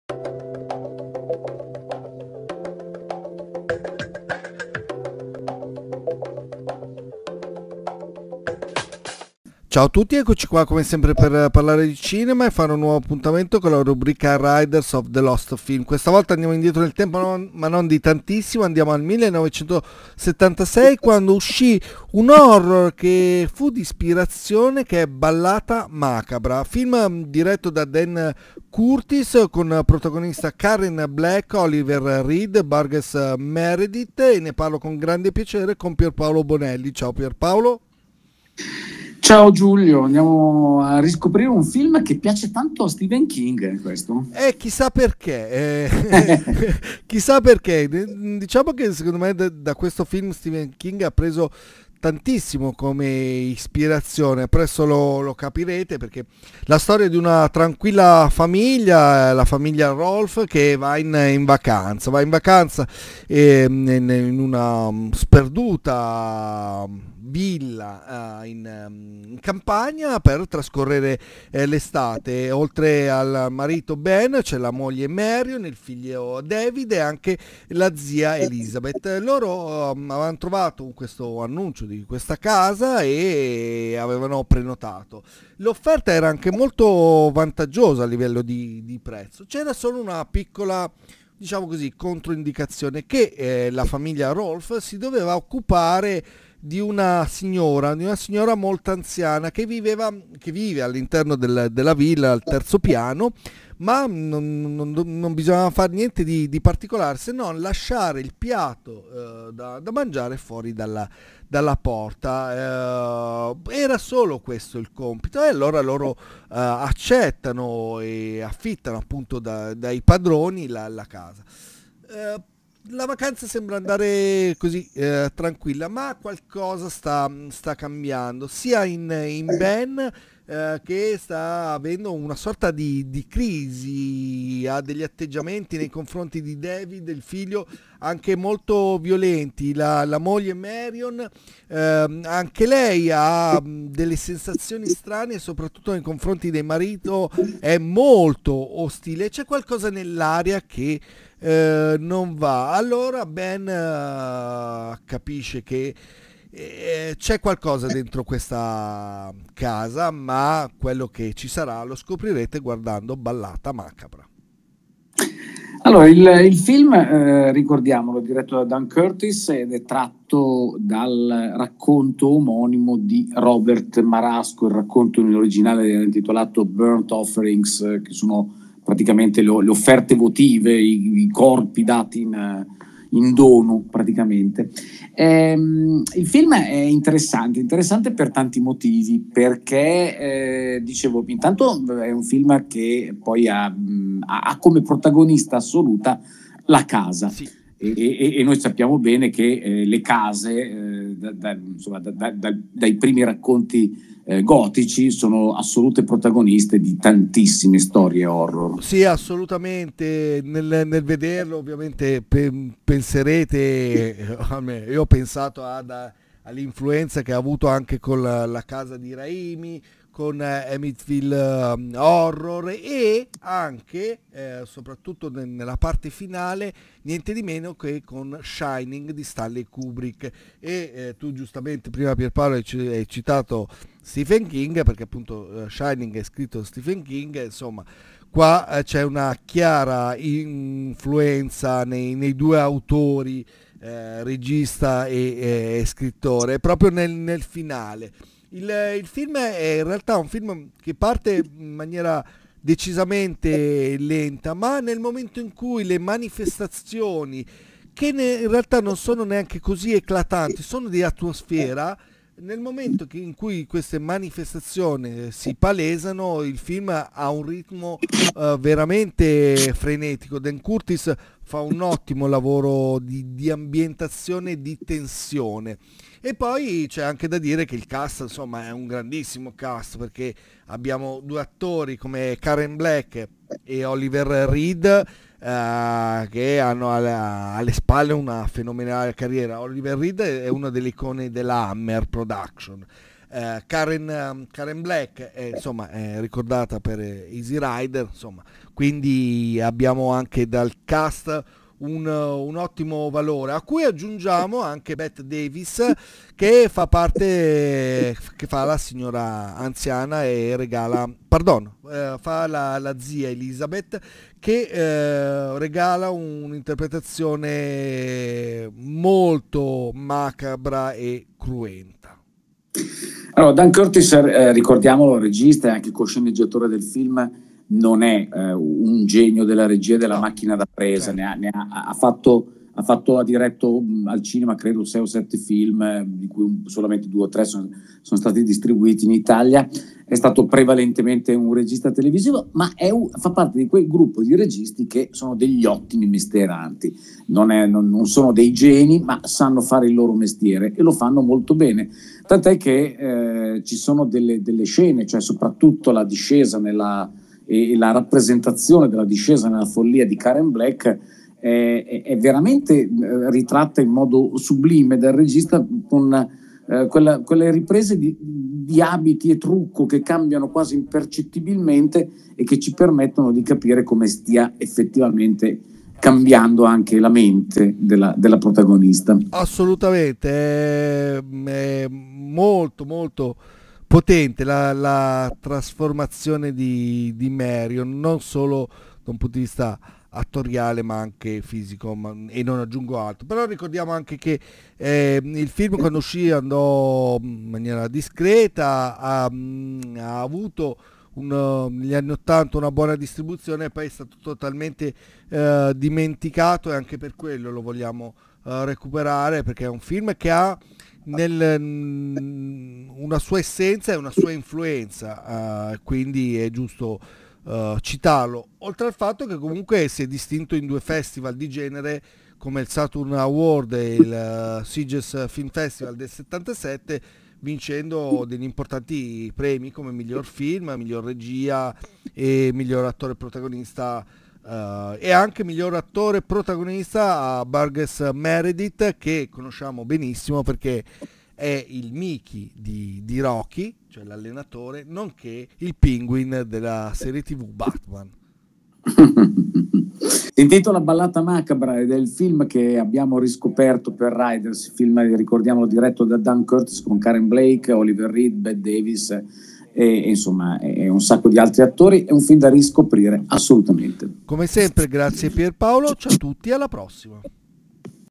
Ballata macabra (1976, Burnt Offerings) di Dan Curtis [Raiders of the lost film]
Per la rubrica "Raiders of the lost film" parliamo di Ballata macabra, horror del 1976 diretto da Dan Curtis con Karen Black, Oliver Reed, Burgess Meredith.